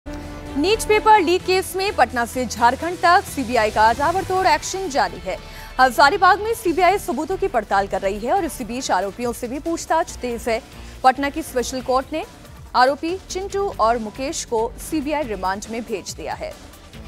NEET Paper Leak Update: पेपर लीक मामले में चिंटू और मुकेश को कोर्ट ने CBI रिमांड में भेजा | News
NEET Paper Leak News: नीट पेपर लीक केस में पटना से झारखंड तक CBI का ताबड़तोड़ एक्शन जारी है. हजारीबाग में CBI सबूतों की पड़ताल कर रही है. इस बीच आरोपियों से भी पूछताछ तेज है.